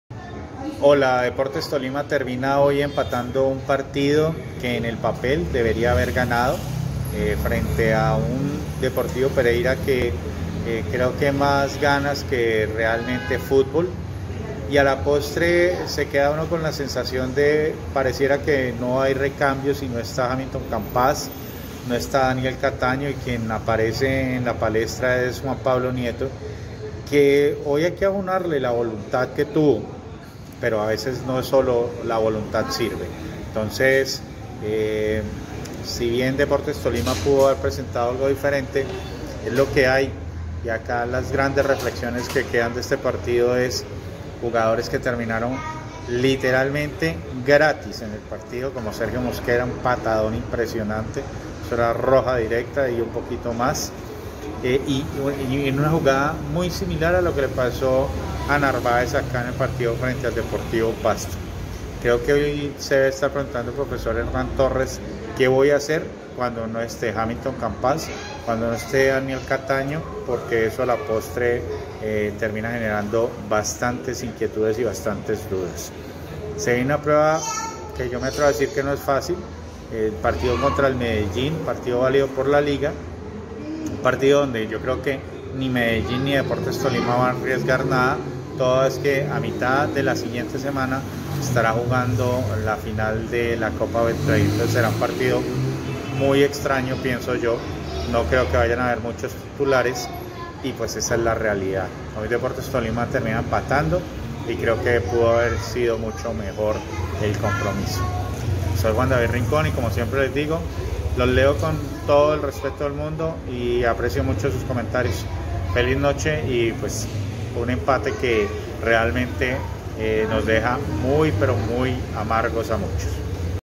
ElRinconDelAnalisis - Un pálido empate en Pereira 🐺🆚🐷
RincónDelAnálisis | Nuestro panelista Juan David Rincon Galindo nos habla un poco acerca de lo que fue el pálido 0-0 entre Club Deportes Tolima SA 🆚 Deportivo Pereira Oficial.

¿Por qué le costó tanto generar fútbol ofensivo a los 'Pijaos'? Déjenos sus comentarios AQUÍ👇